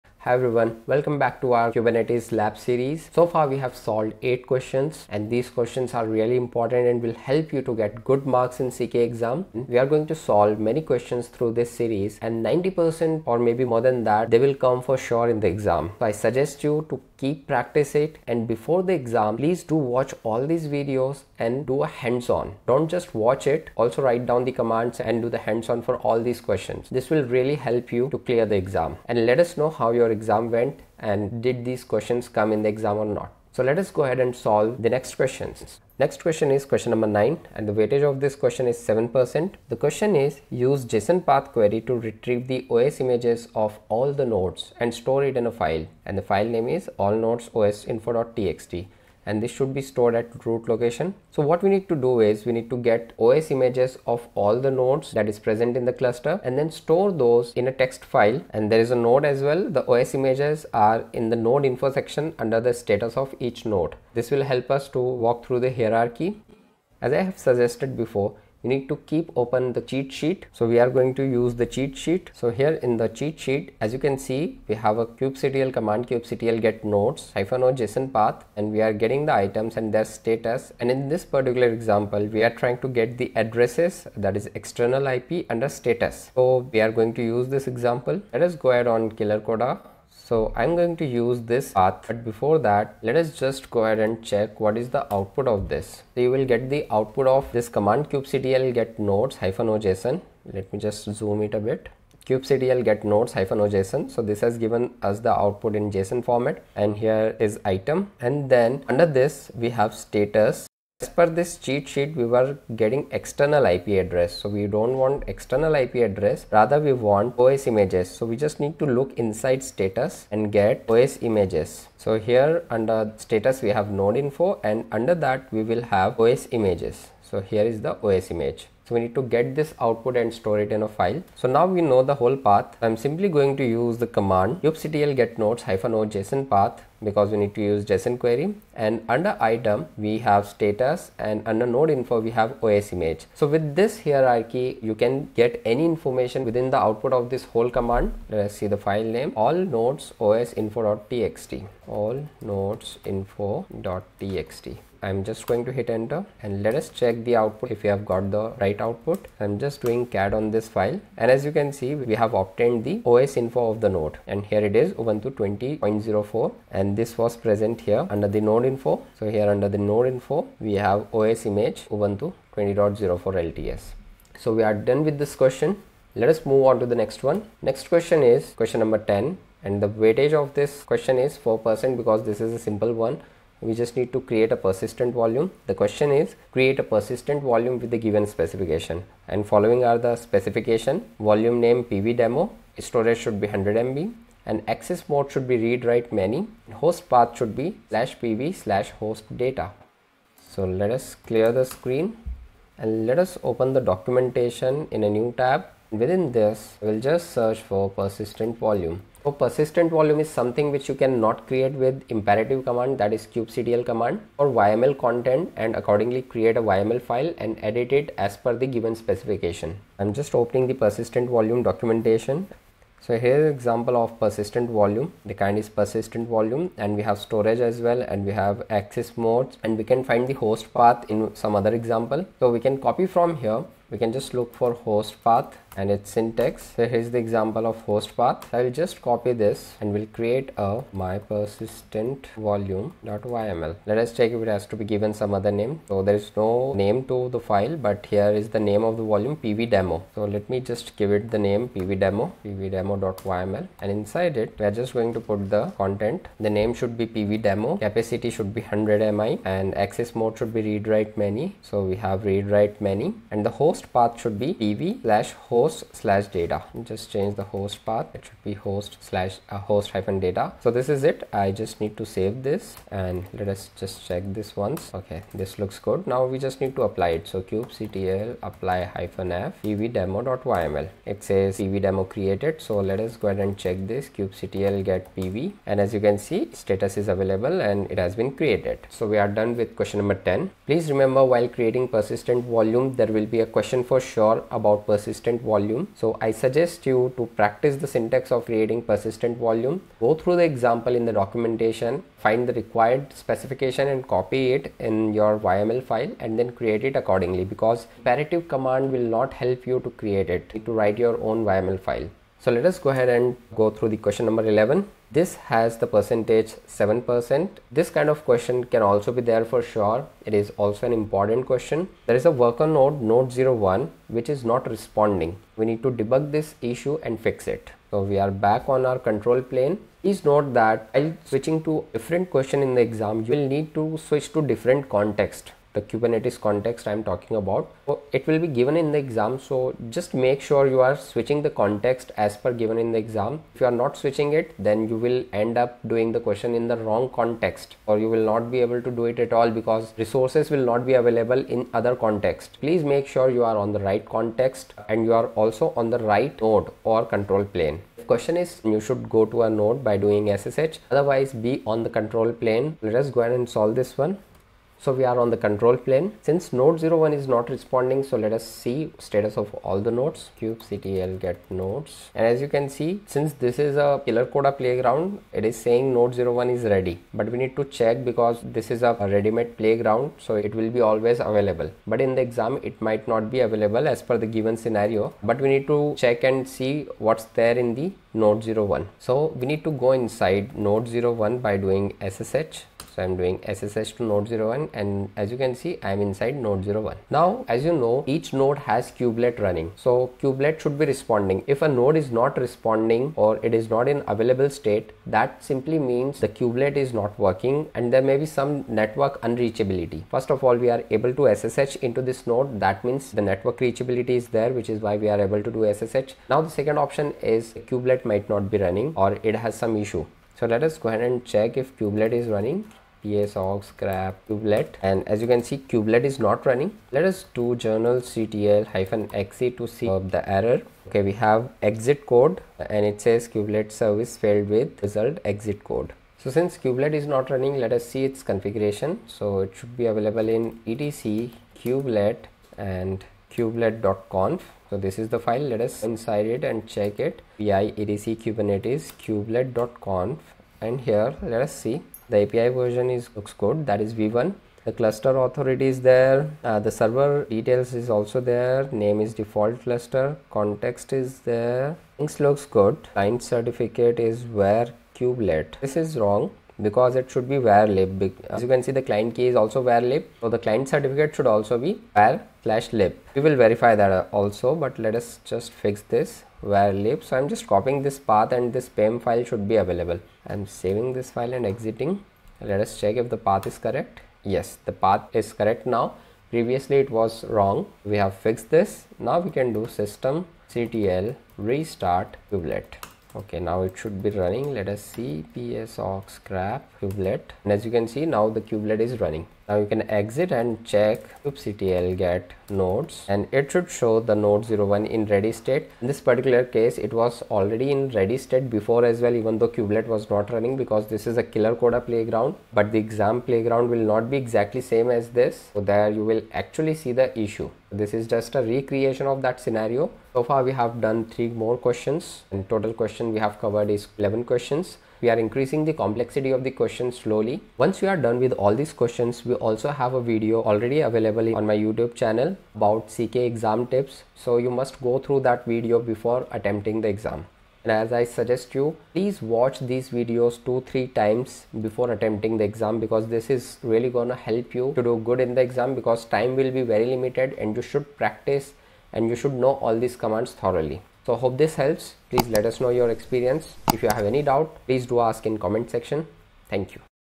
Part 4 | Real CKA Exam Practice Questions | Certified Kubernetes Administrator Mock Exam Guide 2025
Question 11 - Worker Node “node01” not responding, Debug the issue and fix it.​

Question 10 - Create a Persistent Volume with the given specification.​
       Volume Name: pv-demo​
             Storage:100Mi​
Access modes: ReadWriteMany​
  Host Path: /pv/host-data​

Question 9 - Use JSON PATH query to retrieve the oslmages of all the nodes and store it in a file “all-nodes-os-info.txt” at root location.​
Note: The osImage are under the nodeInfo section under status of each node.​

Question 8 - Get the node node01 in JSON format and store it in a file at ​ ./node-info.json​

Question 7 - Create a pod called test-pod in "custom" namespace belonging to the test environment (env=test) and backend tier (tier=backend).​ image: nginx:1.17​

Question 6 - Create a pod called pod-multi with two containers, as given below:​
Container 1 - name: container1, image: nginx​
Container2 - name: container2, image: busybox, command: sleep 4800​

Question 5 - Create a static pod on node01 called static-pod with image nginx and you     have to make sure that it is recreated/restarted automatically in case ​of any failure happens

Question 4 - Deploy a web-nginx pod using the nginx:1.17 image with the labels set to tier=web-app.​

Question 3 - Create a new deployment called my-deployment. Scale the deployment to 3 replicas.             ​
Make sure desired number of pod always running.

Question 2 - Create a new deployment called myproject, with image nginx:1.16 and ​1 replica. Next upgrade the deployment to version 1.17 using rolling​ update​. Make sure that the version upgrade is recorded in the resource annotation.​

Question 1 - Create a new pod called web-pod with image busybox Allow the pod to be able to set system_time​. The container should sleep for 3200 seconds​

This is a video of all the exam questions I have ever asked. If you are preparing to take the CKA, or if you are taking the CKA, then this video is for you. I hope it helps!
This is the first of three videos where I discuss a module of questions from the Certified Knowledge Architect (CKA) exam. These questions provide good examples of what to expect on the exam and how to answer them. There's a ton I could say about the CKA, but in this video I keep it high level and superficially examine the exam to determine what topics will be covered in detail later on.